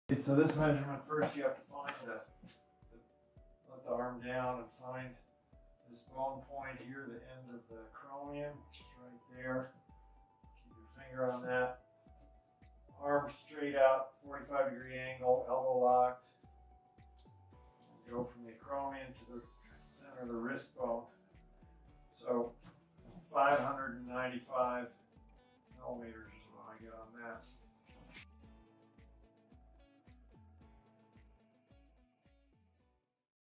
Arm length measurement for Zinn Cycles bike fit
How to measure your arm length for a Zinn Cycles Bike fit or a Clydesdale Bicycles Bike fit.